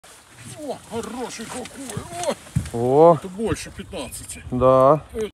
Ponoi river in June, 15lb+ Atlantic salmon before release
Nice Atlantic salmon 15lb+ before release
Guide: Big Dan

Река Поной, июнь. Рыбалка с Биг Дэном. Семга 15фунтов+ перед отпусканием